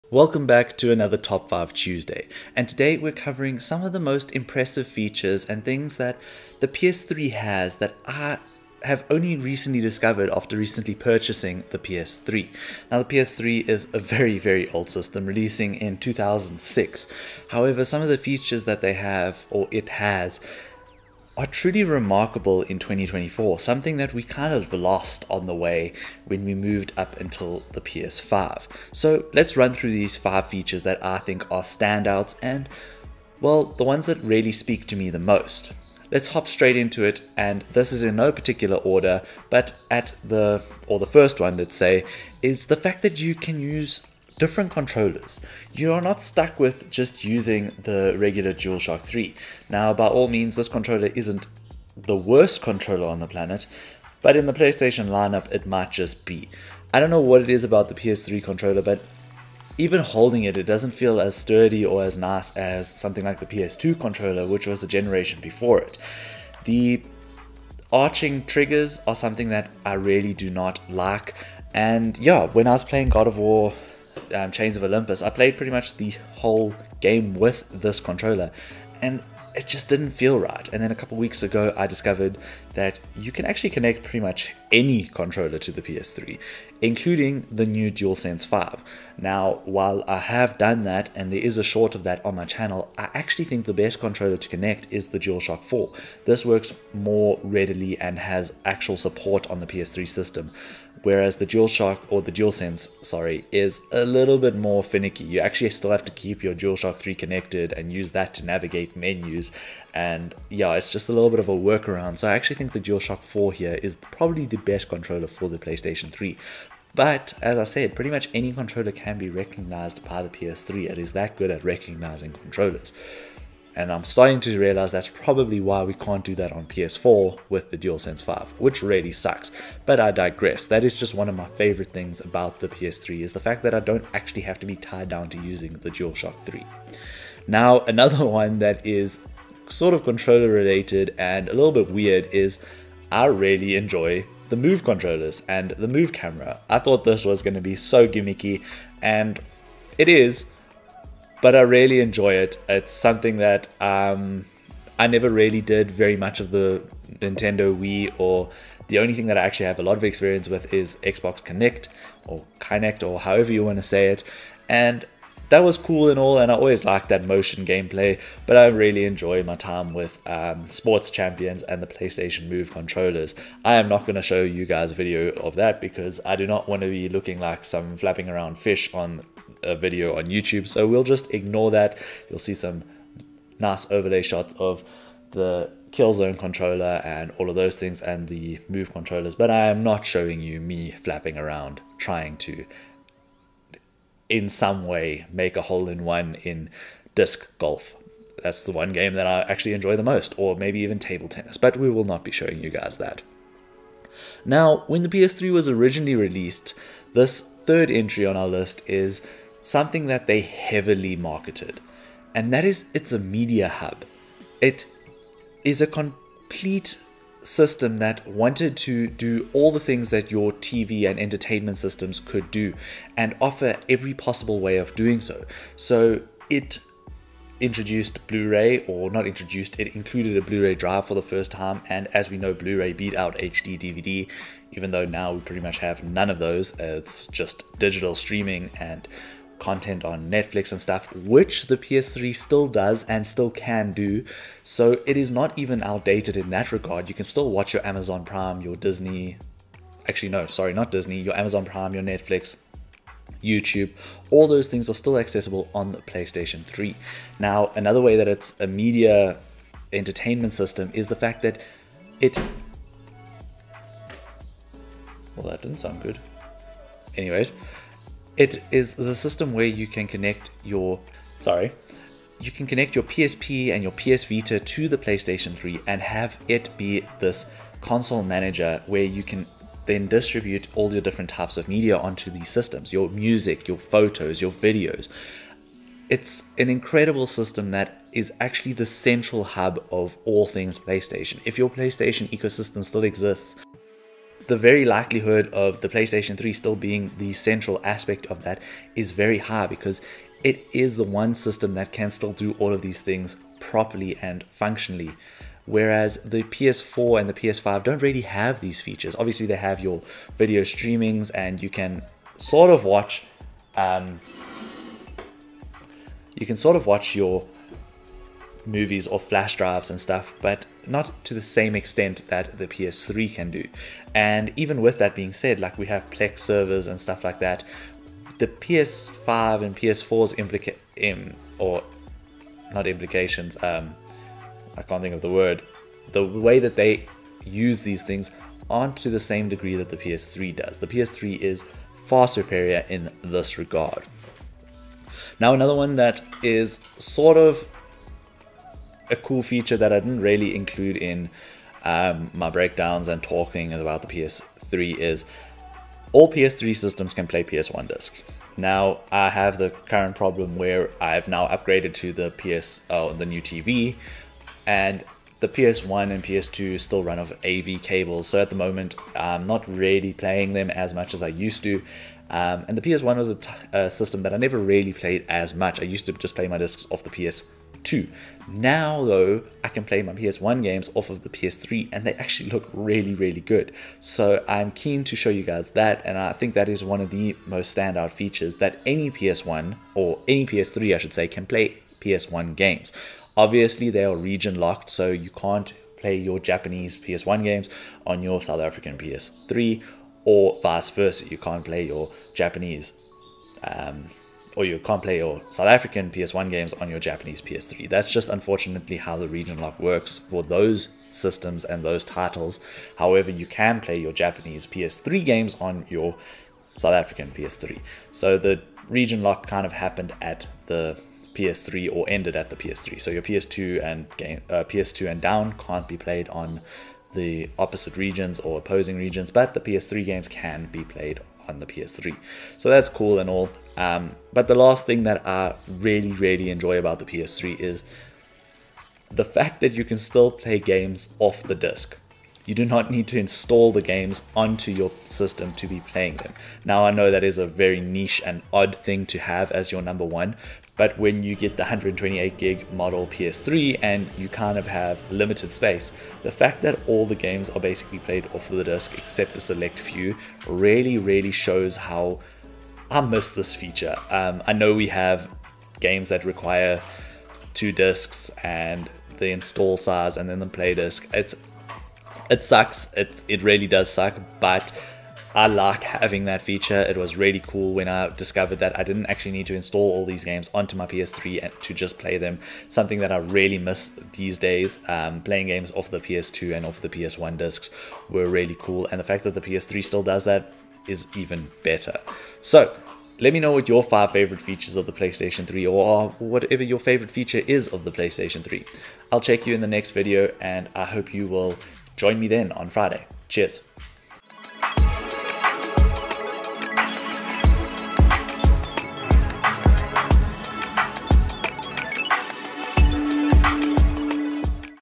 These FIVE PS3 Features are AMAZING in 2024 [T5T #4]
The PS3 has some features that I wish were still around today!